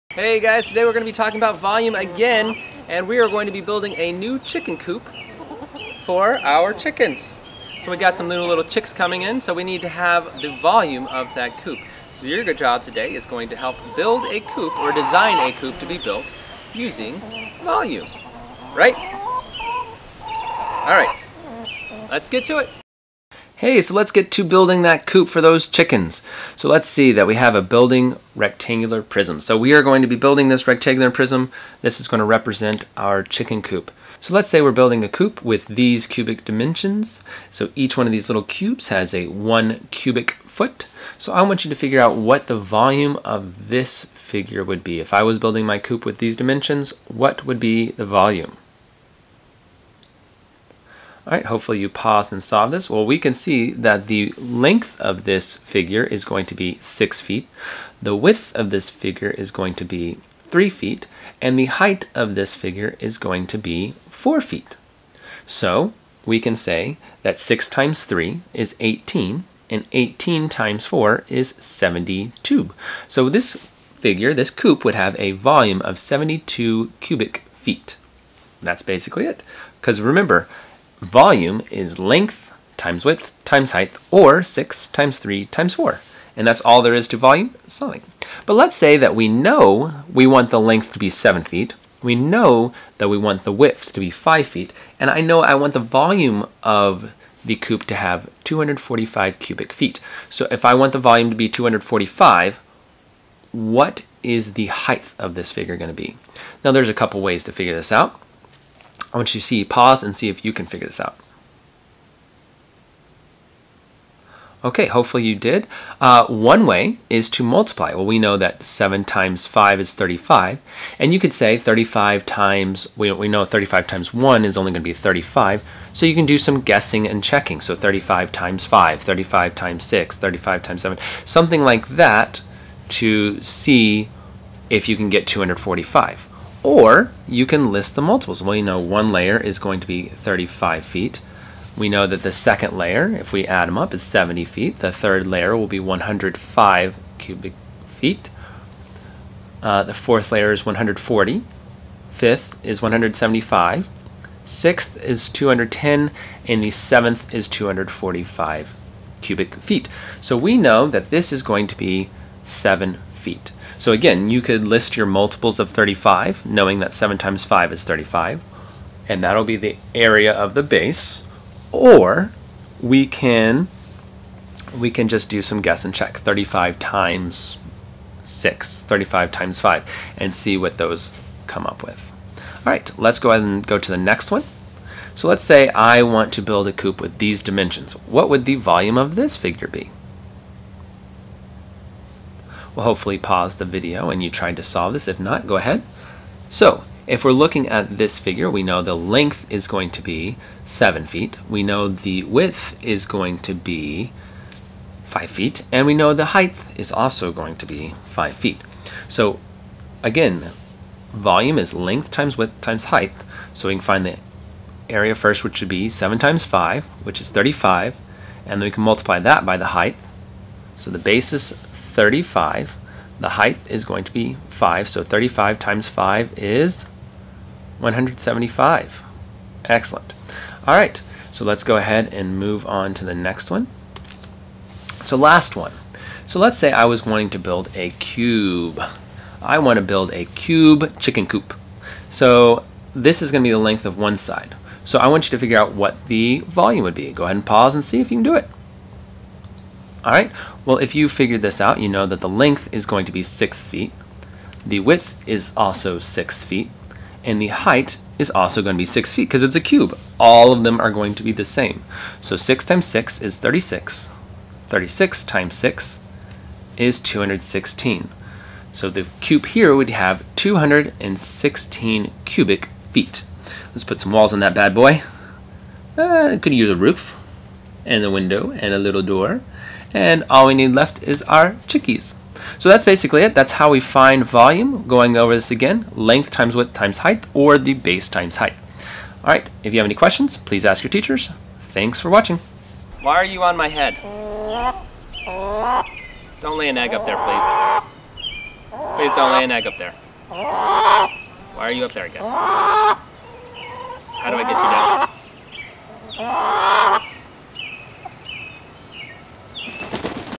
Distant Learning Video Volume Part 2
This lesson covers Volume of rectangular prism again and the different problems that students might be come across in fifth grade.